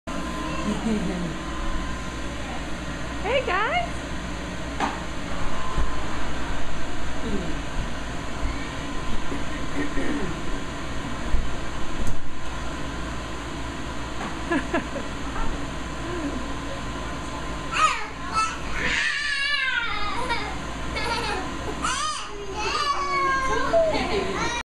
Cruz and his friends in the bounce house
Cruz and his friends hanging out in the bounce house